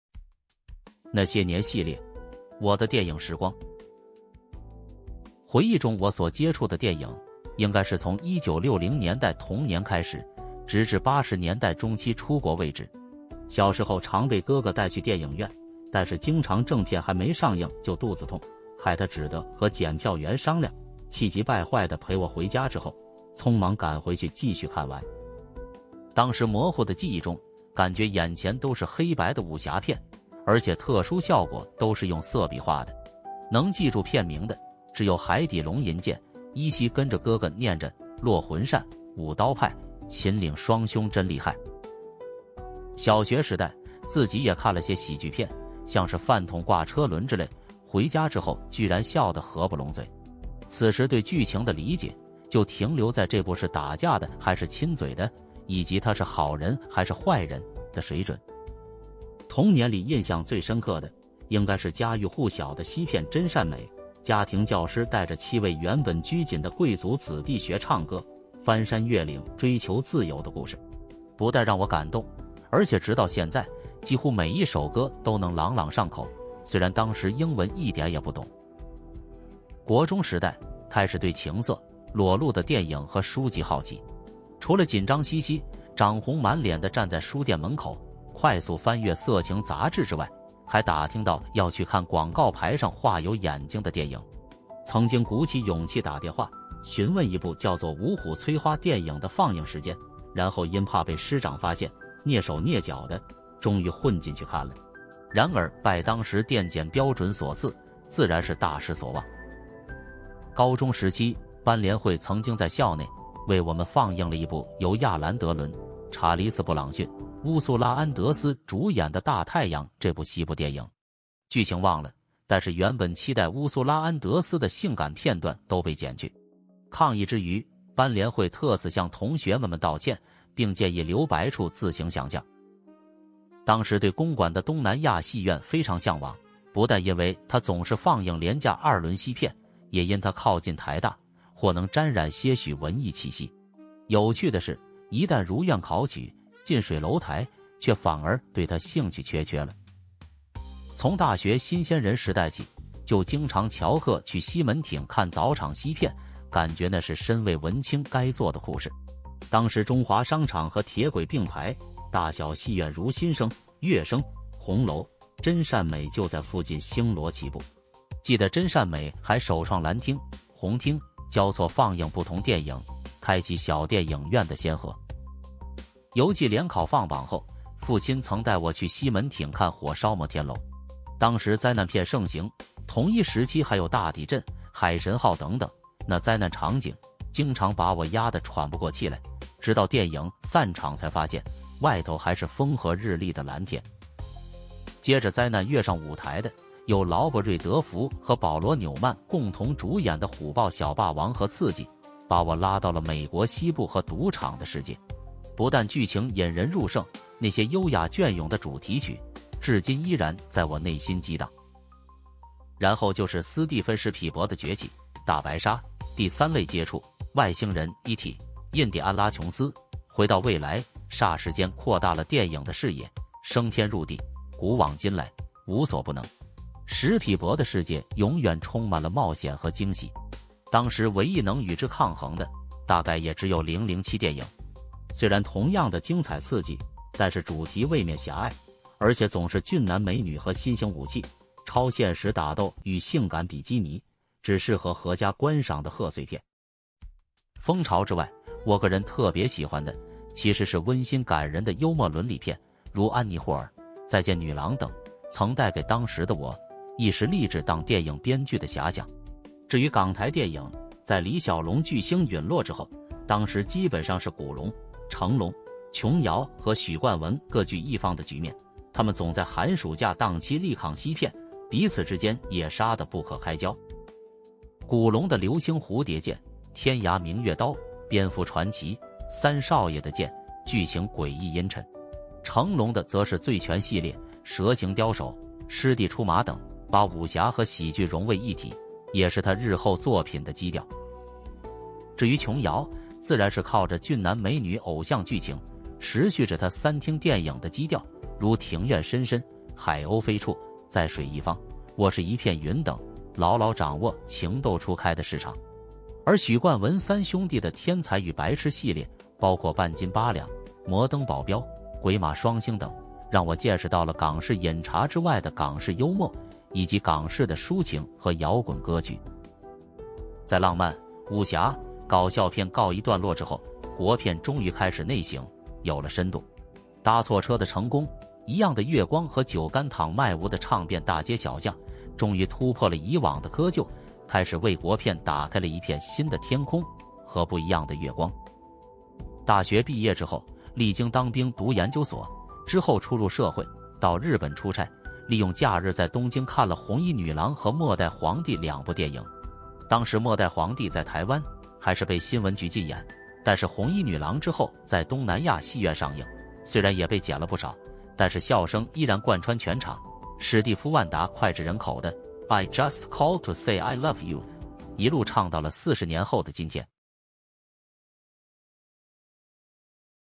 那些年系列：我的電影時光
回憶中我所接觸的電影，應該是從1960年代童年開始，直至80年代中期出國為止。
小時候常被哥哥帶去電影院，但是經常正片還沒上映就肚子痛，害他只得和剪票員商量，氣急敗壞地送陪我回家之後，匆忙趕回去繼續看完。
當時模糊的記憶中，感覺眼前都是黑白的武俠片，而且特殊效果都是用色筆畫的。能記住片名的，祇有「海底龍吟劍」，依稀跟著哥哥唸著：「落魂扇，五刀派，秦嶺雙兄真厲害。」
小學時代，自己也看了些喜劇片，像是「飯桶掛車輪」之類，回家之後居然笑得閤不攏嘴。此時對劇情的理解，就停留在「這部是打架的還是親嘴的？」以及「他是好人還是壞人？」的水準。
(故事內容純屬虛構 圖片示意皆非本人)